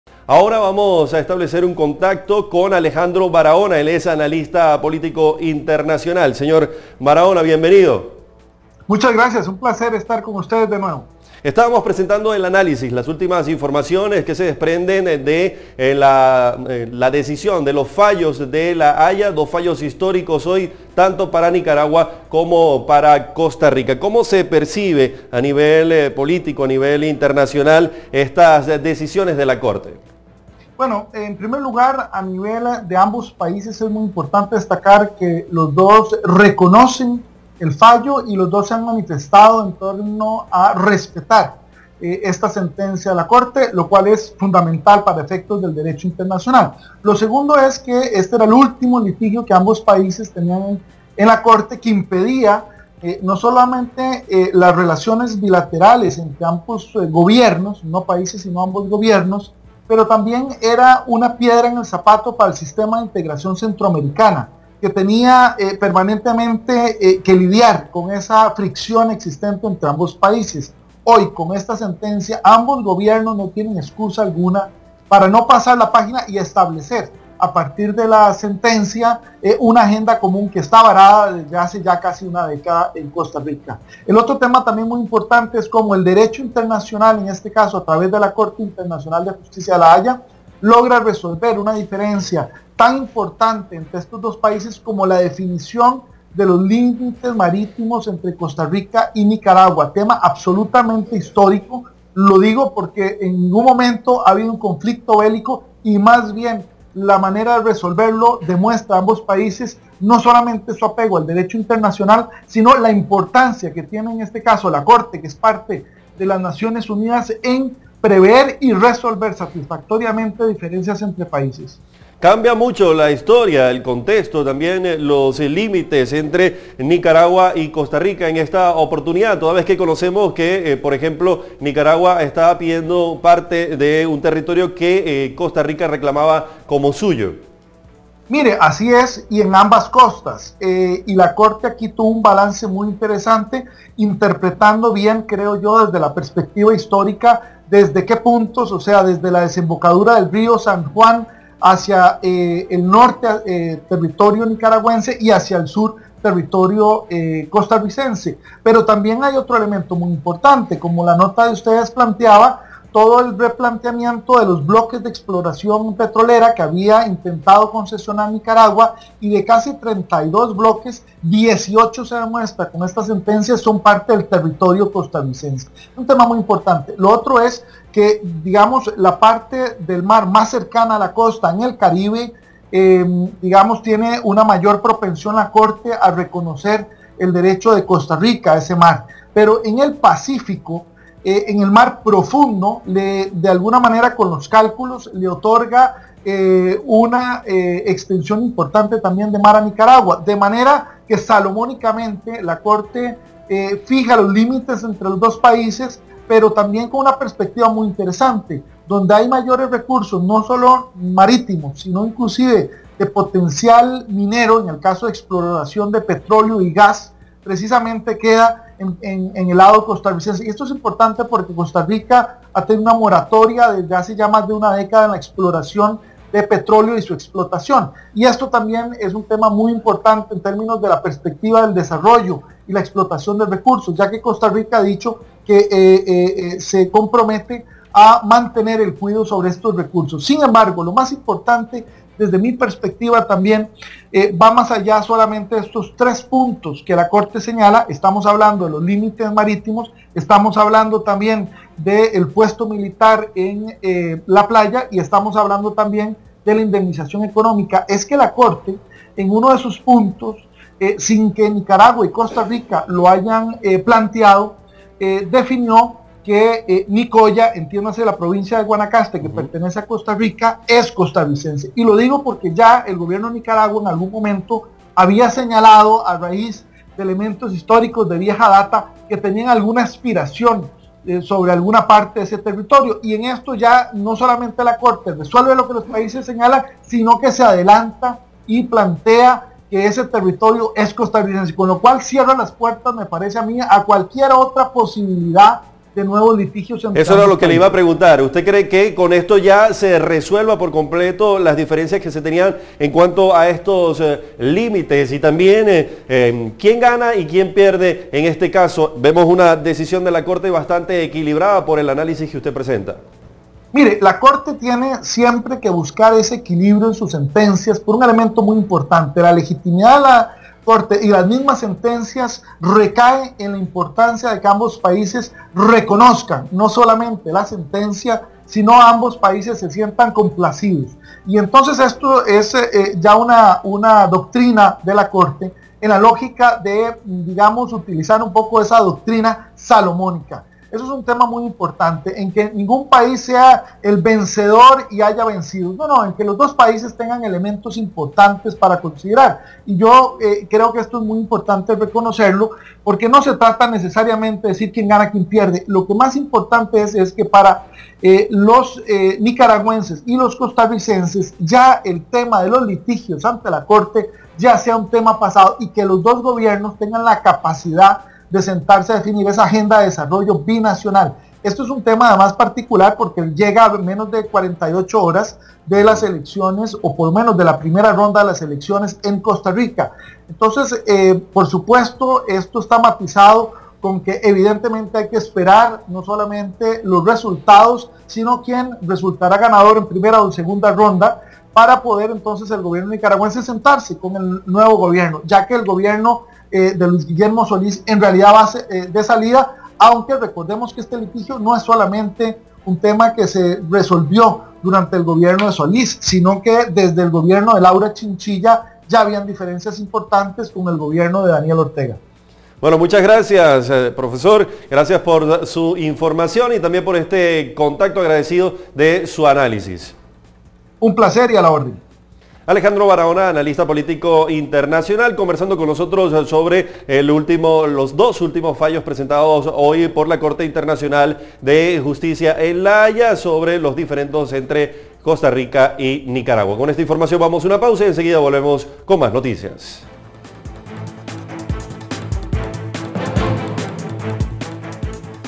Corte Internacional emite fallos equilibrados en diferendos Costa Rica-Nicaragua
El analista político internacional, Alejandro Barahona, calificó de muy equilibrados los fallos que emitió este viernes 2 de febrero, el pleno de magistrados de la Corte Internacional de Justicia en La Haya, sobre el caso de los límites marítimos y el caso de la compensación que deberá pagar Nicaragua por la controversia sobre los humedales, a Costa Rica.
El experto indica que a nivel de ambos países es importante destacar que " los dos reconocen el fallo y los dos han manifestado que respetaran la decisión de la corte, aspecto que es fundamental para el derecho internacional".
También hace referencia a que este era el último diferendo que mantenían ambas naciones, que entorpecían las relaciones bilaterales, y bloqueaba los avances en el Sistema de Integración Centroamericana, "hoy con esta decisión los dos gobiernos no tienen excusa alguna para no pasar la página y establecer a partir de la sentencia una agenda común que esta varada desde hace más de una década",.